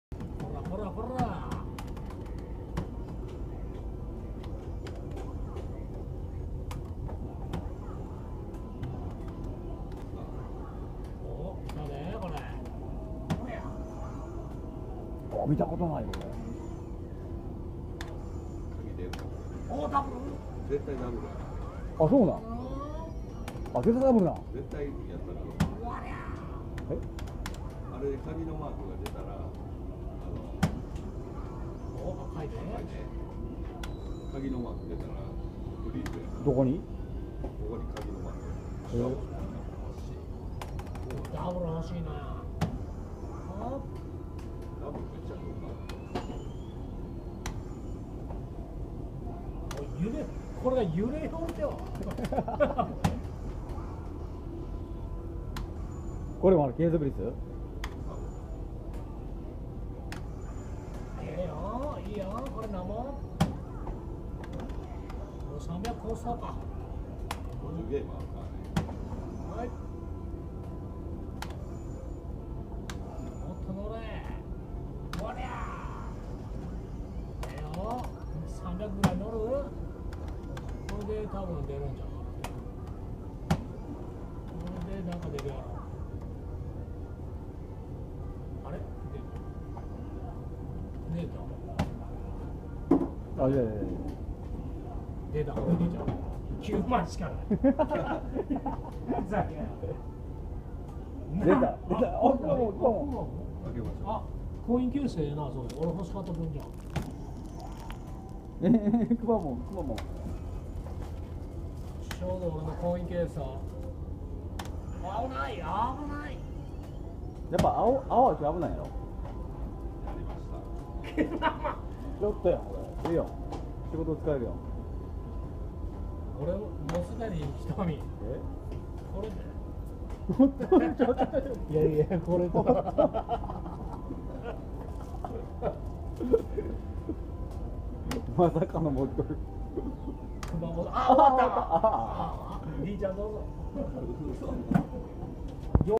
あかつき丸でパチスロAKB打ってみたくまモンGET(;ﾟДﾟ)
あかつき丸でパチスロAKB打ってみた動画です。